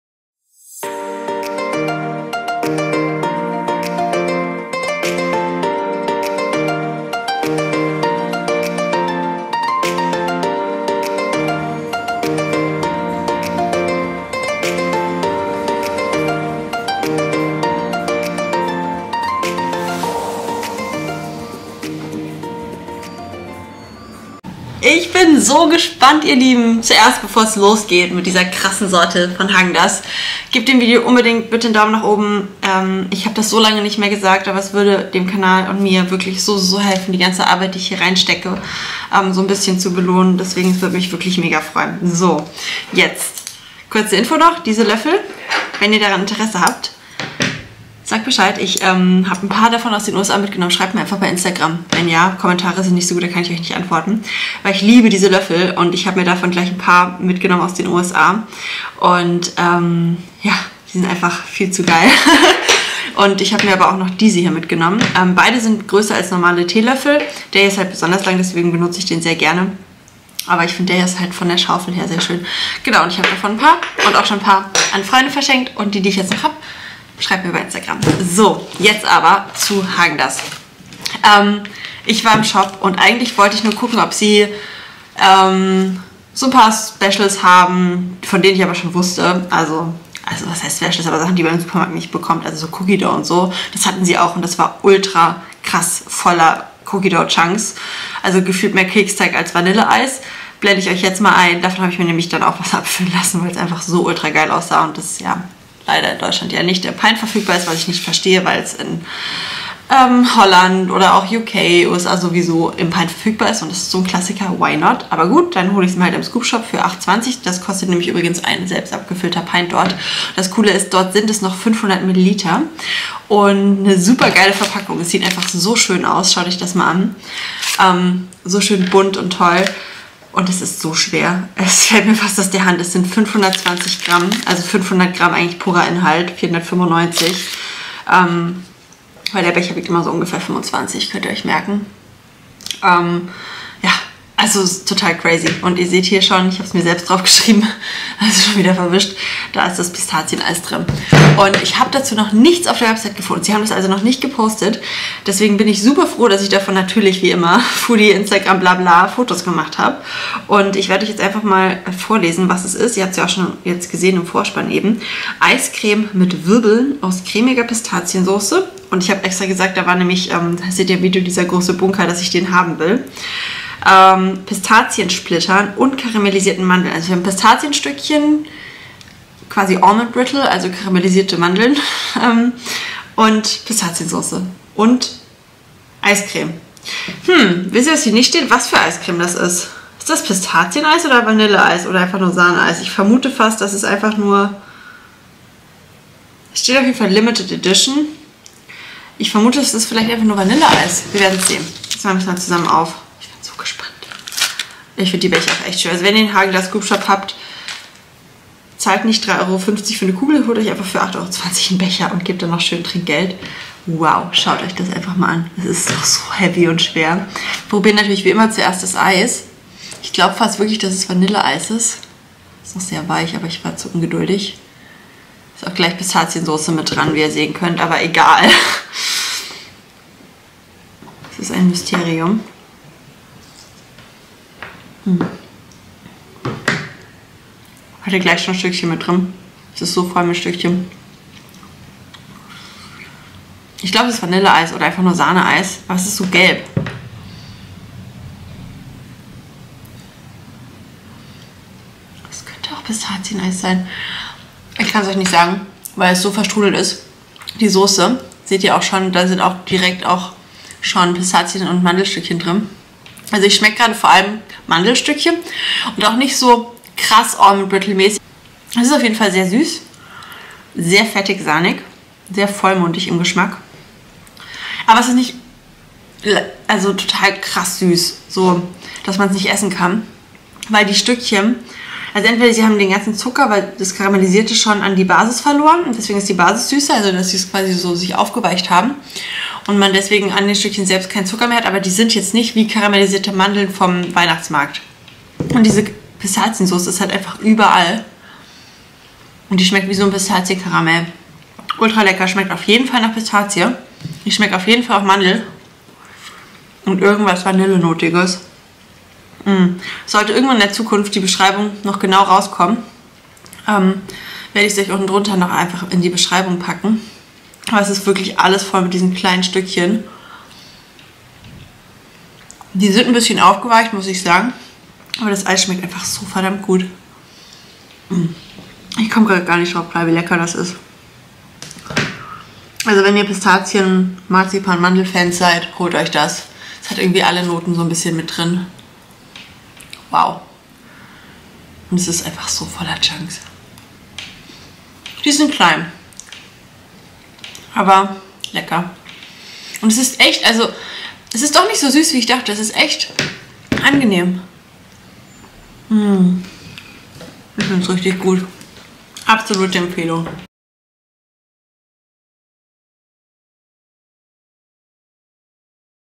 NEU! PISTACHIO & CREAM EIS Special Sorte| HÄAGEN DAZS | TEST
Es ist so eine Floskel hier, ich weiss... Aber es hilft uns Creatoren wirklich SOOOO immens! xoxo

⇨ FACTS:
• Preis: 8,20€ im Scoop Shop am Kudamm
• macros Pro Pint (495g/520ml)  unbekannt!
• Eiscreme mit Wirbeln aus cremiger Pistaziensauce, Pistaziensplittern und karamellisierten Mandeln

⇨ WEITERE EISCREME REVIEWS:
🎥 ALLE HÄAGEN DAZS VIDEOS:

***Vielen lieben Dank, dass ihr euch dieses Video angesehen habt.